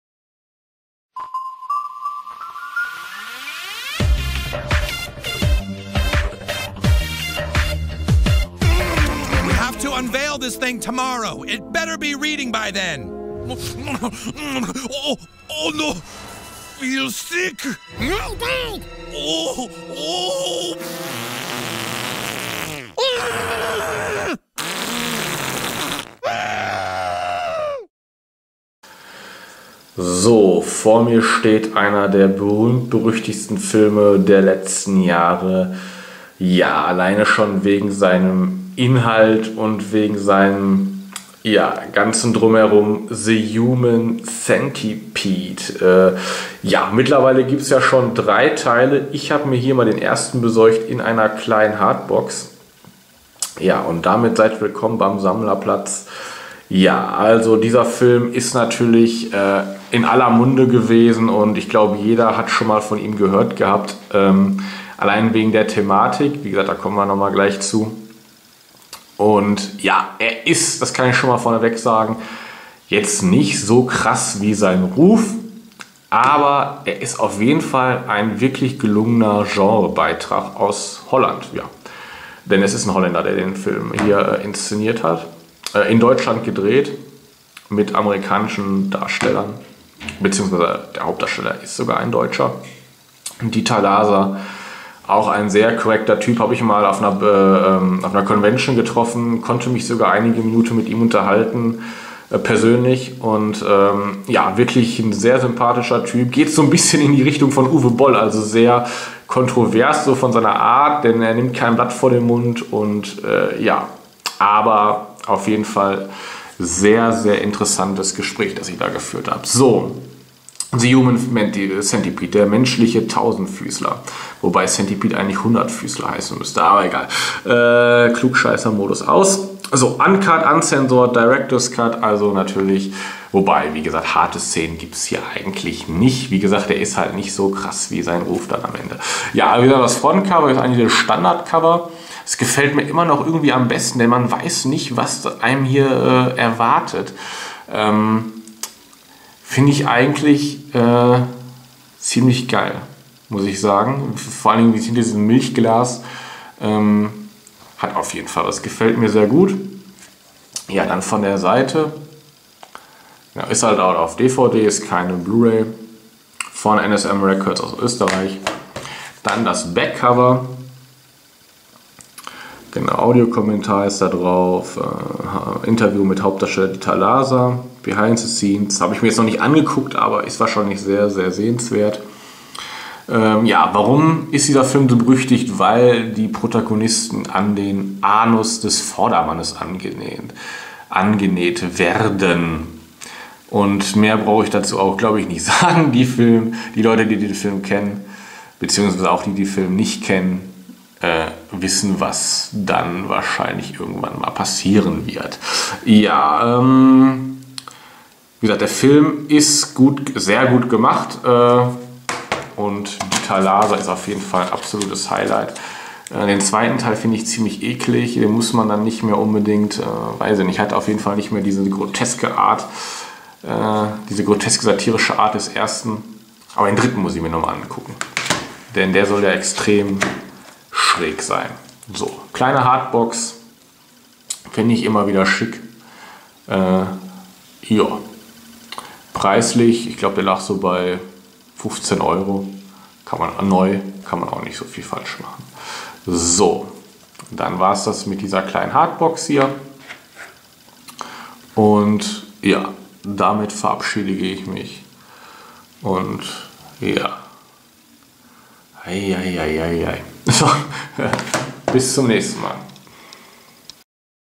Sammlerplatz: Folge 75 - The Human Centipede (Hartbox)
Tja die Idee hätte auch aus einer South park Folge stammen können. Einer der großen "Skandal" Filme der letzten Jahre. Krank? Ja schon. Grotesk? Ja auch. Und klar ist der Ruf immer krasser als das Endergebnis. Aber trotzdem ist der Film alles andere als schlecht!
und dazu 100% medizinisch akkurat!